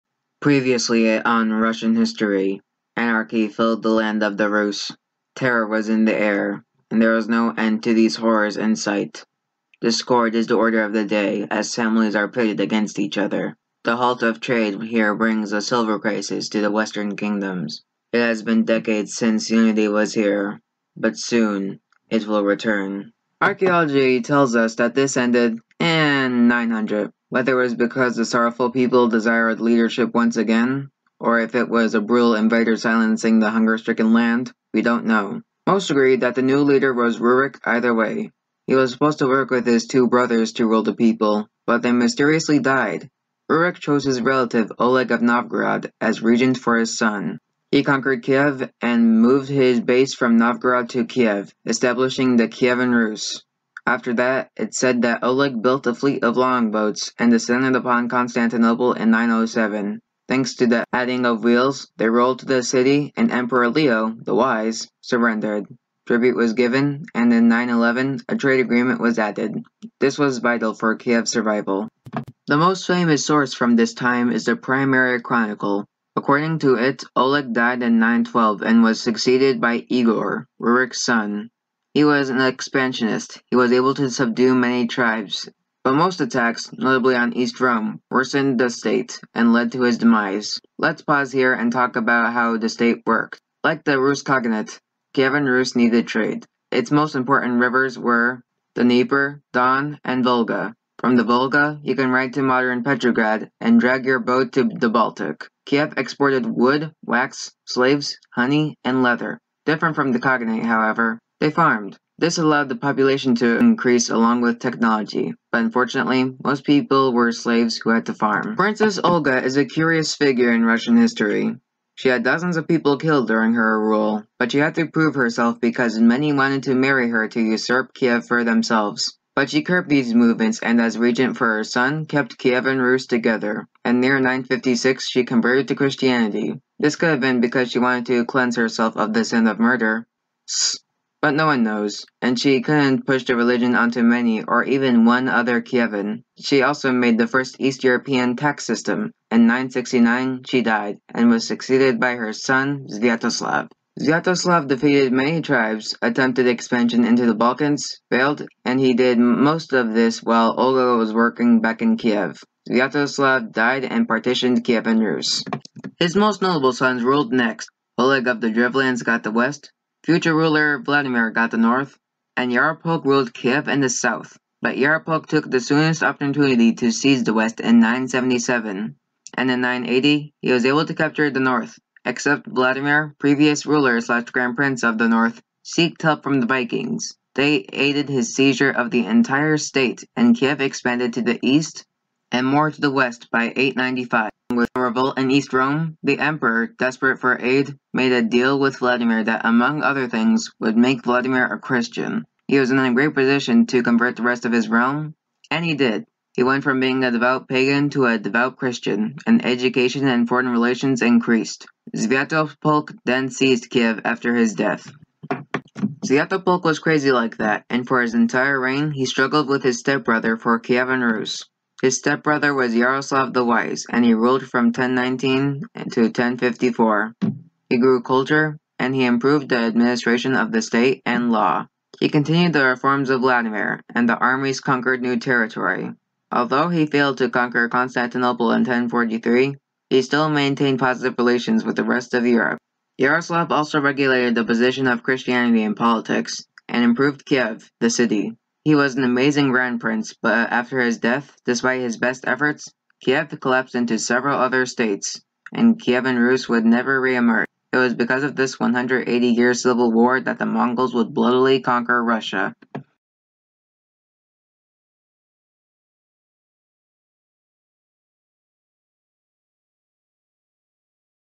Today's Topic: Kievan Rus
After the loose, unstable Rus' Khaganate, Kievan Rus would be the kingdom that the Eastern Slavs would call home. It had many ups and downs, but through it all it left an important legacy for Russia, Belarus, & Ukraine. Also I'm sorry if the maps aren't 100% accurate.
Europe with Countries - Single Color by FreeVectorMaps.com
Sources:
Rus’ Khaganate-New World Encyclopedia;
Rurik|Norse Leader|Britannica.com;
How Did Russia Begin? (KnowledgeHub);
Oleg of Novgorod|History of Russia; Rurik House-English Abstract; Igor|Russian prince|Britannica.com;
Ancient Rus: History of Russian trade and crafts:: Russia-InfoCentre;
Russia, the Kievan Rus, and the Mongols: Crash Course World History #20 ;
Princess Olga of Kiev-Russiapedia History and mythology;
Sviatoslav I|prince of Kiev|Britannica.com;
Epic History: Russia Part 1 (Epic History TV);
Yaroslav the Wise|prince of Kiev| Britannica.com